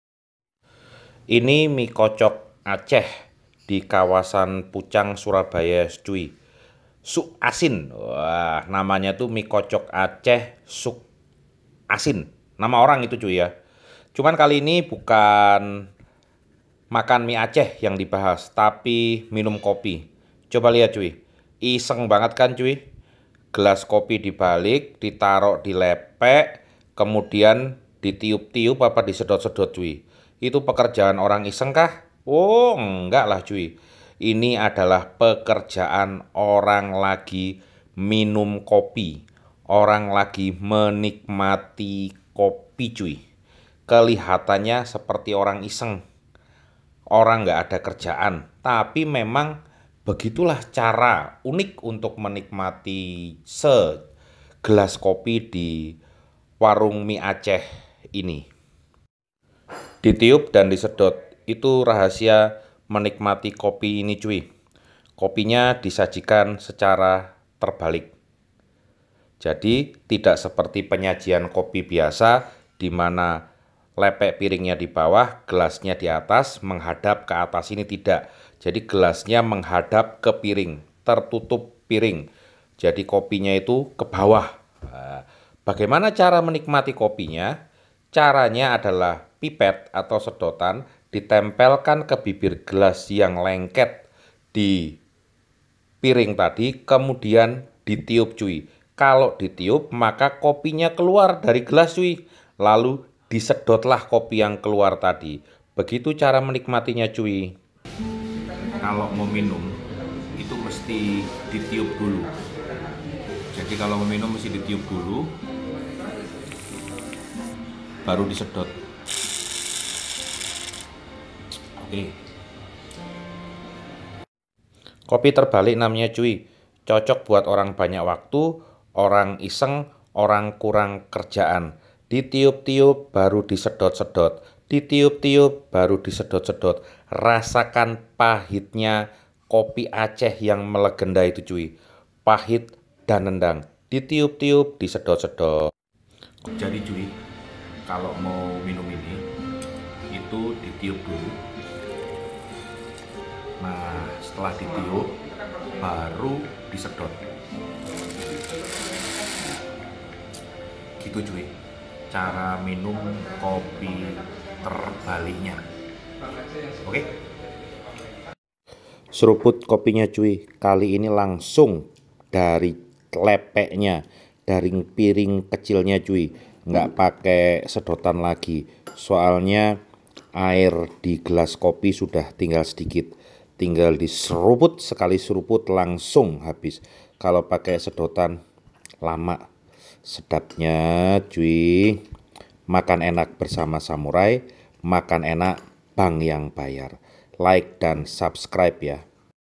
ANEH !!!! MINUM KOPI ACEH DENGAN GELAS TERBALIK DI SURABAYA ???
Sekarang sudah bisa Merasakan Sensasi Menyeruput Kopi Aceh dengan Gelas Terbalik di Kota Surabaya | Sensasi Menyeruput